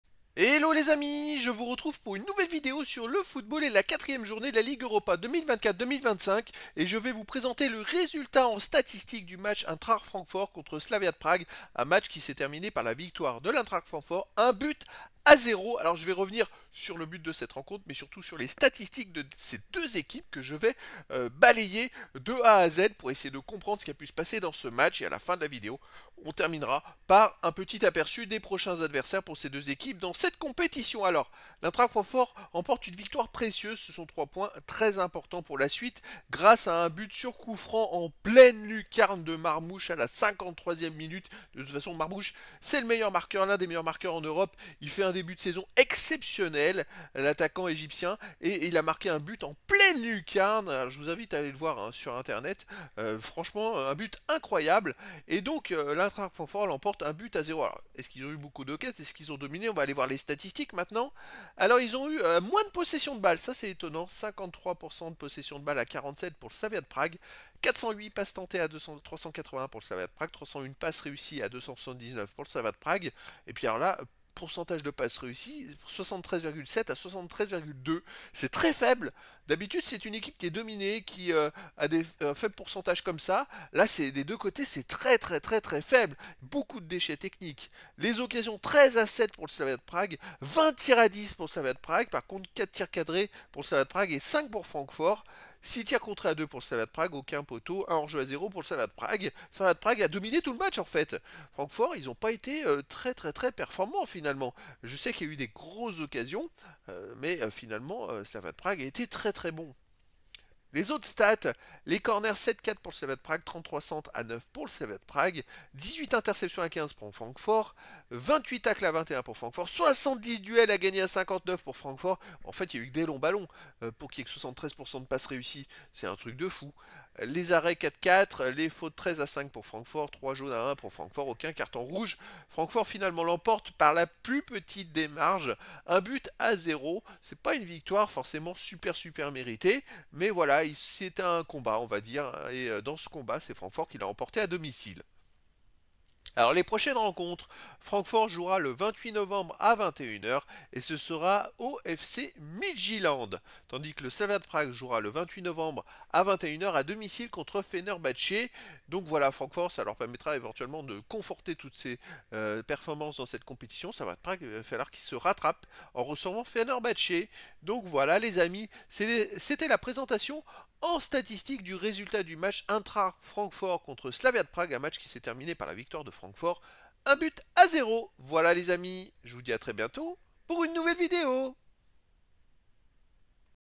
EINTRACHT FRANCFORT - SLAVIA PRAGUE : 1-0 - 4ème journée - Ligue Europa 2024/2025
Je vous présente le résultat en statistiques du match de football du 07/11/2024 et de la 4ème journée de la Ligue Europa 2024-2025 : Eintracht Francfort contre Slavia Prague.

La nouvelle formule de Ligue Europa comprend un seul groupe de 36 équipes et 8 journées de championnat.

Je vous présente le résultat du match avec les buteurs et de nombreuses informations.

Je vous communique toutes les statistiques des 2 équipes pour découvrir le scénario du match et la tendance.

Je termine la vidéo par le calendrier des 2 équipes et les informations sur le prochain match à jouer.